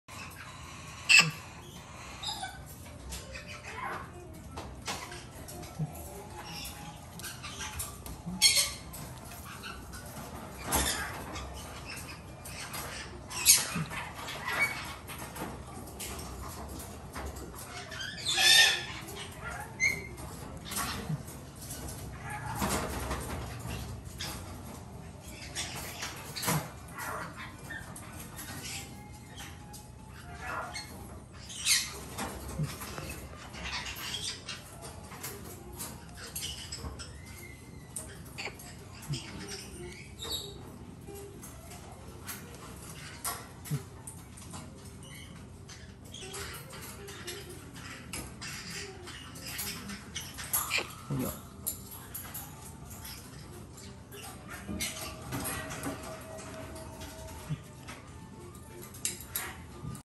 FREAK ヒメコンゴウインコ 雛入店 販売中
ヒメコンゴウインコ　男の仔の雛が入店しましたので、お譲り致します。体重は約300g、コミドリコンゴウインコの約2倍で、少し大きめの中型コンゴウインコをお探しの方には、まさにぴったりのサイズではないでしょうか。現在、さし餌 3回、とても可愛い仔です。